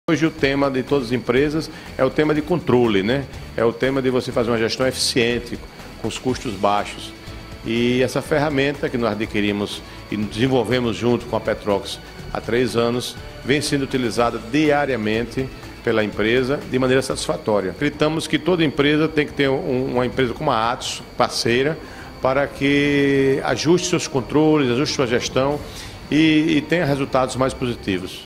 GRUPO PETROX também utiliza o CardServices, conciliador de cartões da Atos Capital
Você ainda não usa um conciliador de cartões? Conheça quem já usa e quais os benefícios!

GRUPO PETROX também utiliza o CardServices, conciliador de cartões da Atos Capital. Com a palavra, Alvaro Neto diretor comercial.